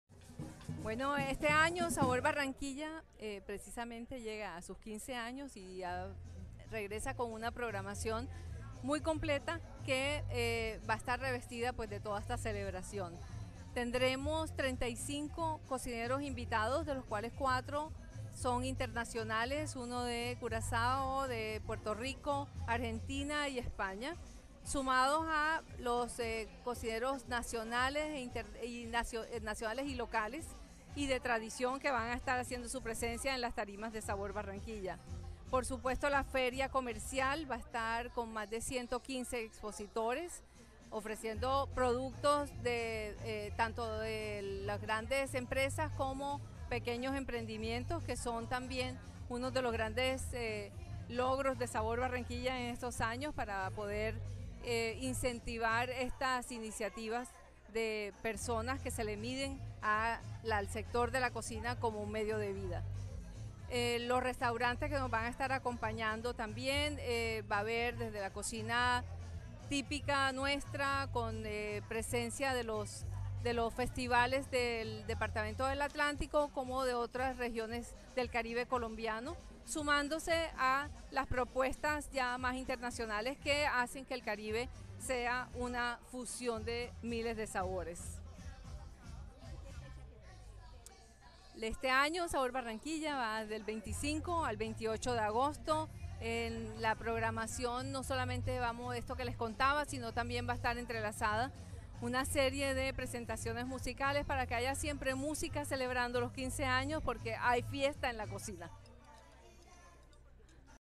Sabor Barranquilla, 15 años celebrando nuestra gastronomía Caribe
La versión número 15 de Sabor Barranquilla se llevará a cabo entre el 25 y 28 de agosto, en Puerta de Oro, Centro de Eventos del Caribe.

En esta edición, Sabor Barranquilla tendrá como tema central ‘Cocinas indígenas: un diálogo pendiente’, en el que conoceremos su aporte a la gastronomía a través del uso de ingredientes ancestrales.

El evento contará con la participación de 32 chefs, entre internacionales, nacionales y locales, 11 investigadores y conferencistas, 115 expositores de productos y servicios de la industria gastronómica y 35 restaurantes que deleitarán a los comensales en la gran plaza de comidas.